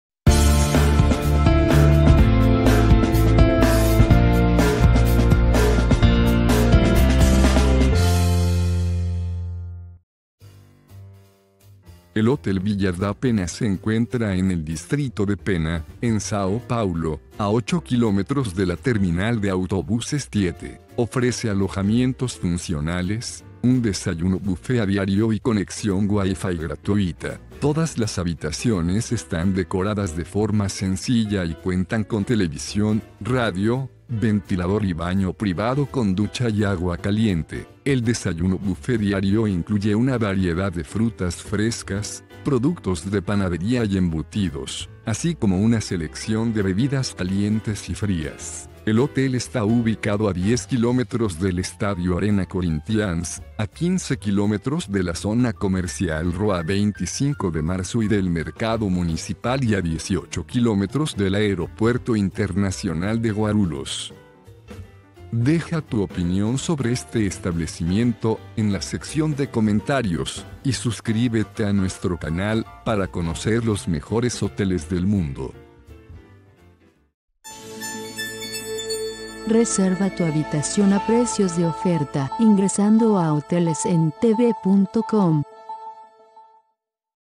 Hotel Villa da Penha en Sao Paulo
Hotel Villa da Penha otorga alojamiento 2 estrellas a quienes visitan Sao Paulo. La construcción se encuentra en Avenida Amador Bueno da Veiga, 564.